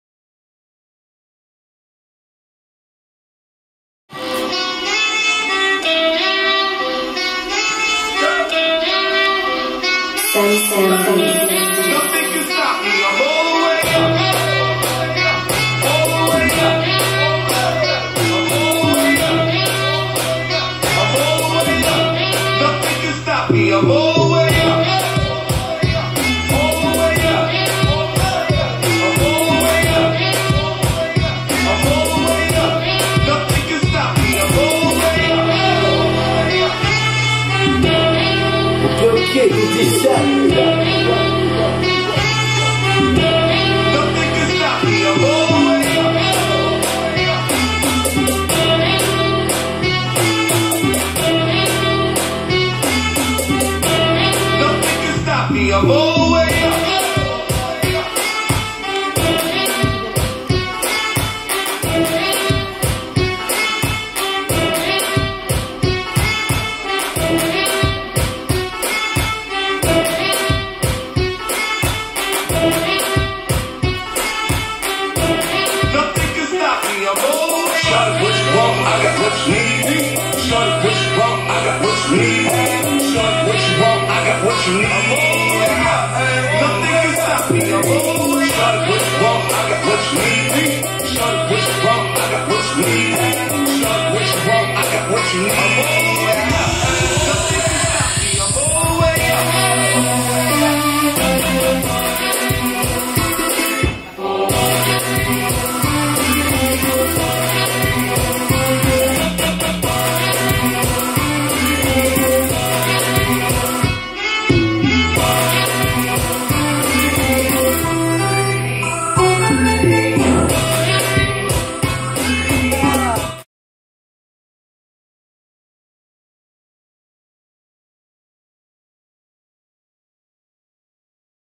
Ken Golitin (Secret Team), Tarraxhinha DEMO, (Karmøy, Norway)
Artist : Ken Golitin & the Secret team, Paris
Class :  Demo after Tarraxhinha class

Karmøy, Norway, Mars 11, 2017
Organizer: Eli Ann Smelvær